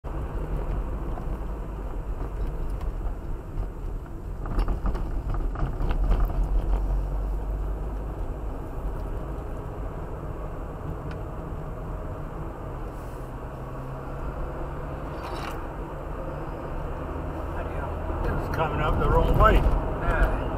dangerous driver - Bisham Roundabout on A404
This happened on Mon 26 August 2024 at around 10pm.
(ignore the date/time on the video - the dashcam clock is wrong)